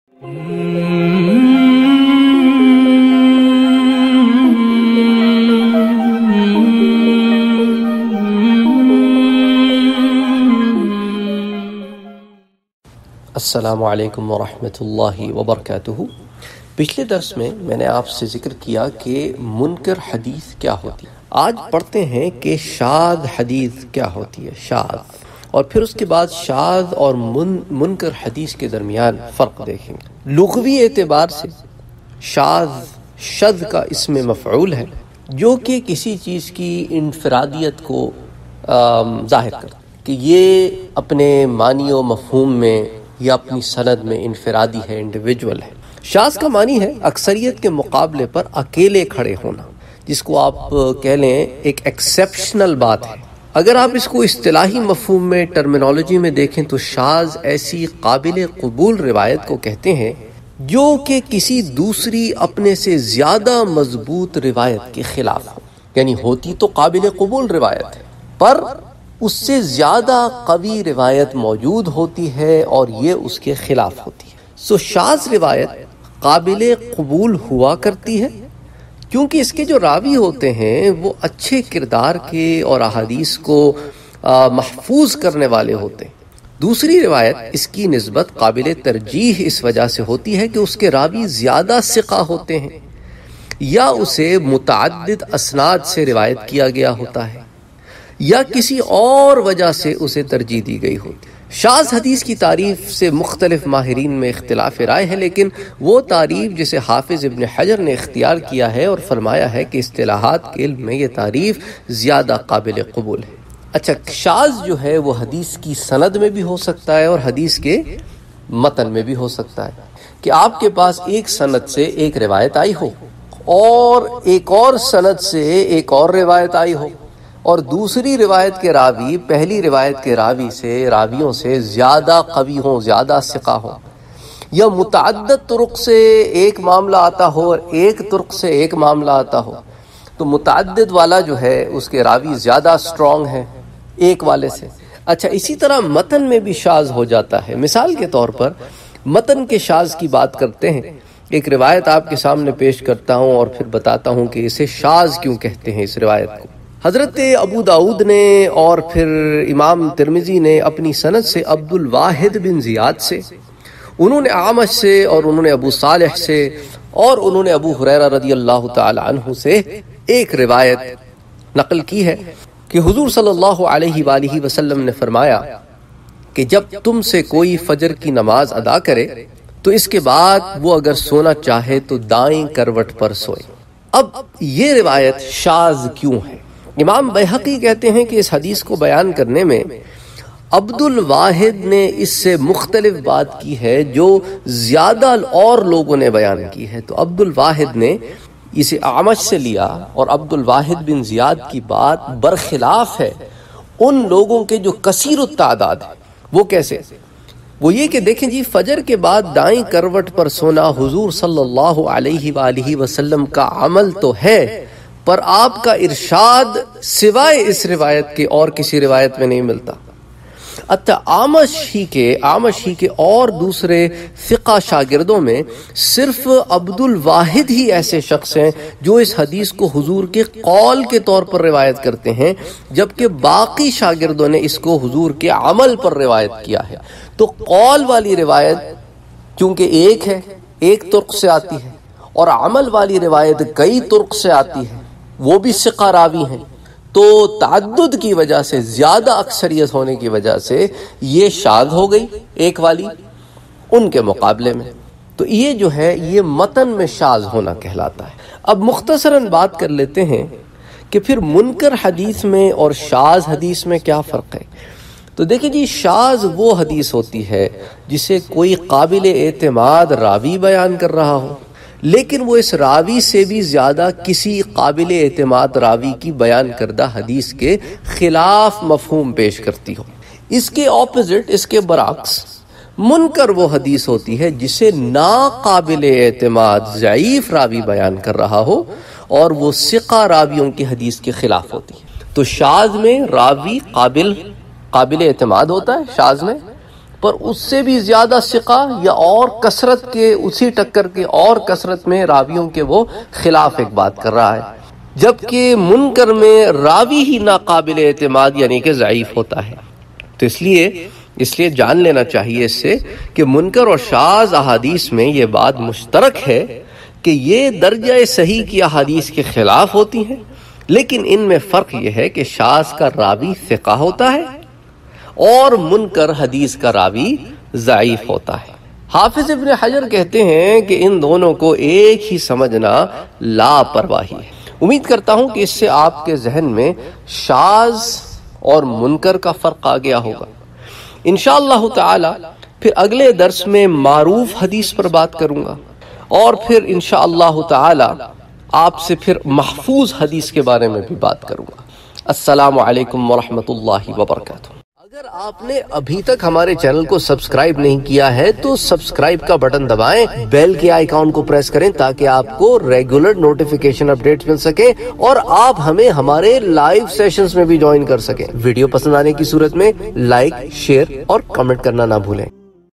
Shaaz hadith ka matlab | Shaz aur Munkar mein Farq | ILM ul Hadith | Darse Nizami Tutorial
Shaaz hadith ka matlab